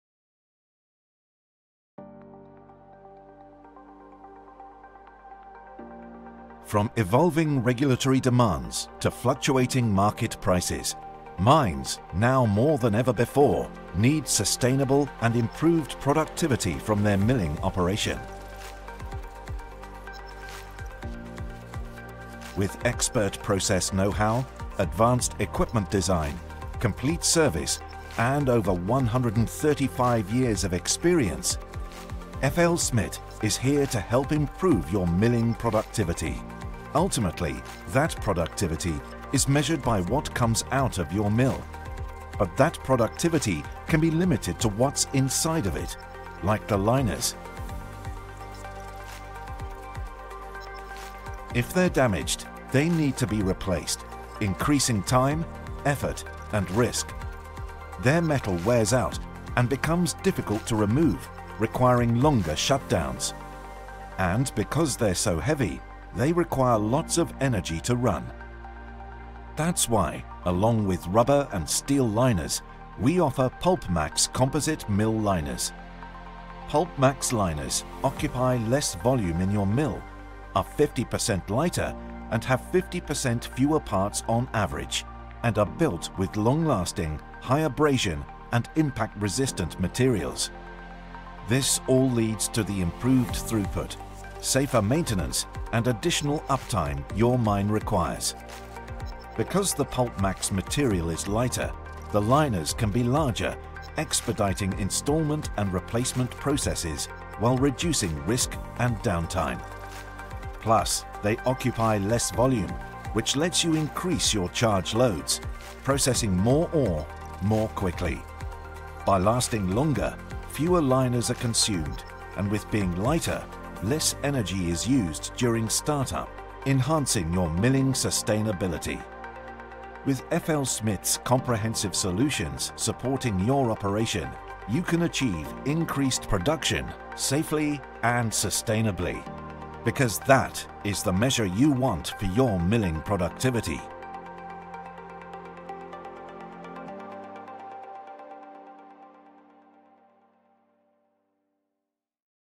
PulpMax™ Композитные мельничные футеровки | FLSmidth
Высокопроизводительные футеровки мельниц полусамоизмельчения и шаровых мельниц для всех видов обработки полезных ископаемых. Узнать больше